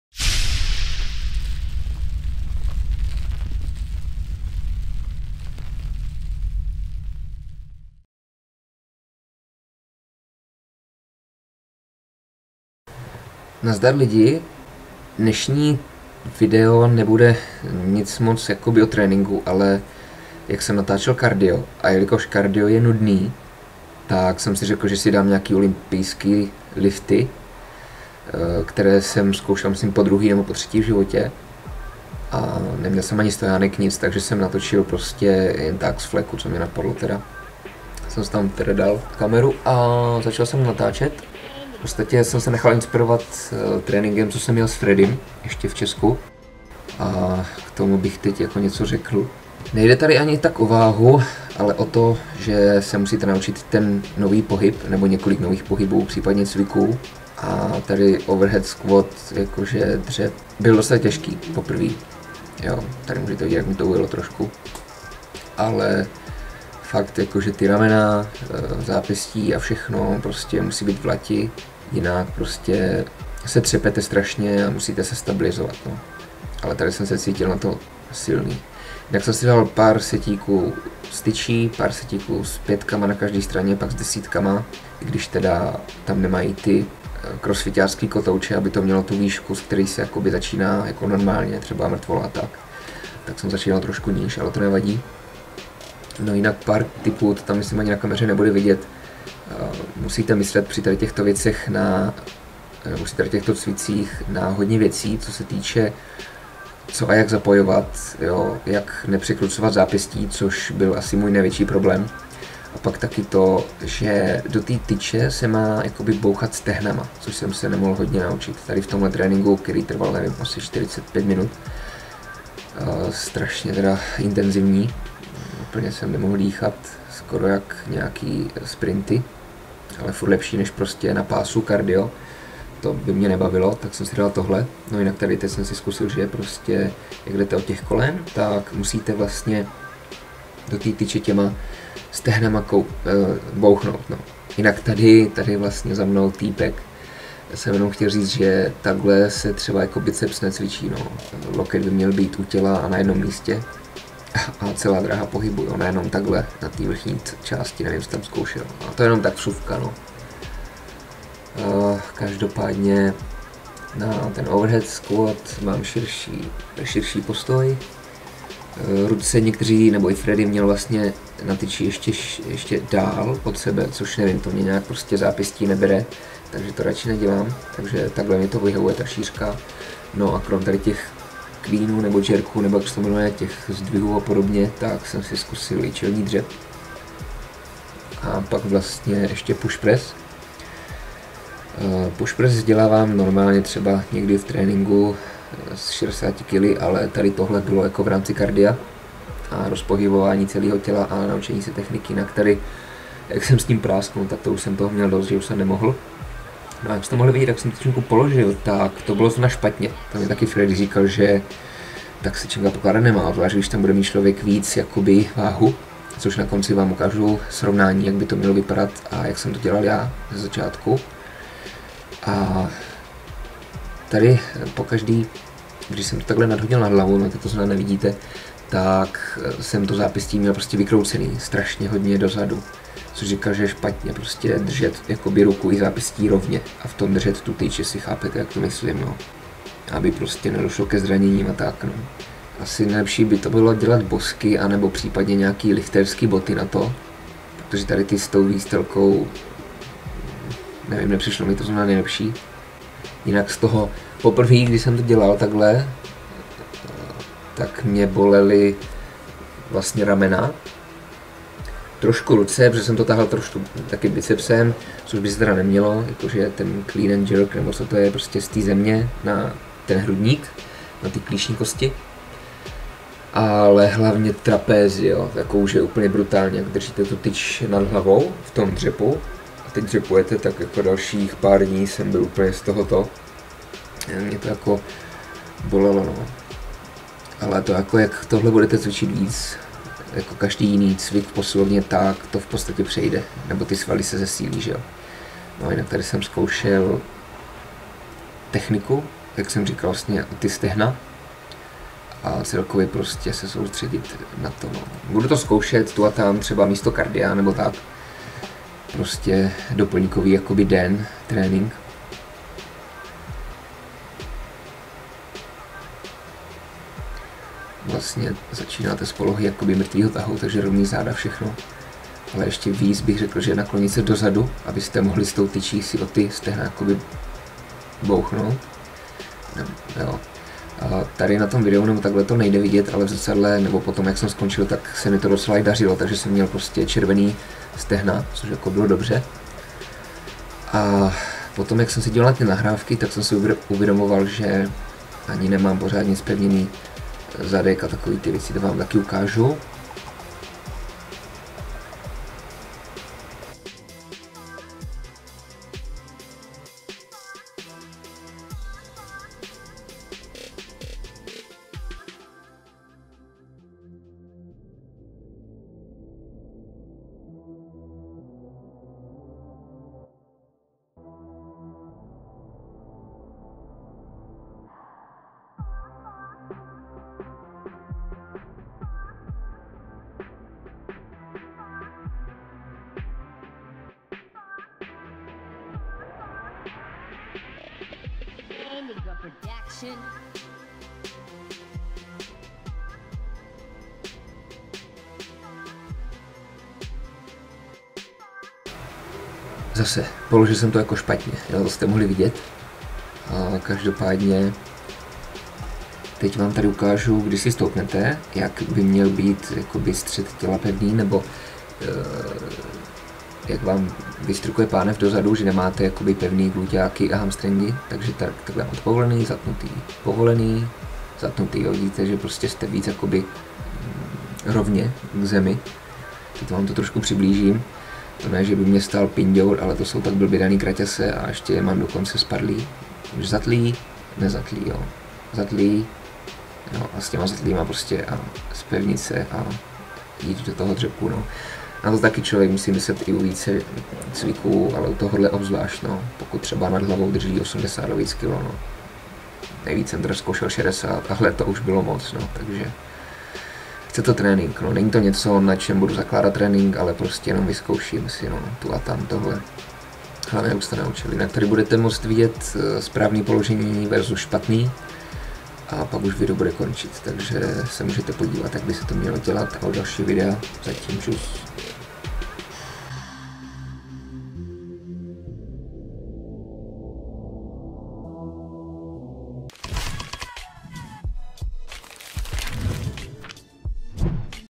Trénink s Fredym | začínám crossfit - no gains? | jak netrénovat biceps | správné pokládání činky
Další zajímavá videa na Kanálu ;)

[PŘIDEJ SE a klidně se ptej na co potřebuješ!]

[VIDEO EDIT]
LG